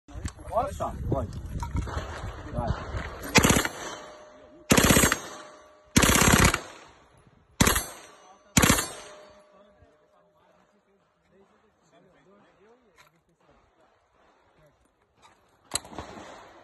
Taurus smt 9 mm iat gustavo atirando
Vídeo de Léo Damaceno iat gustavo zanetti atirandobno cemtro de treinamento tatico da cbc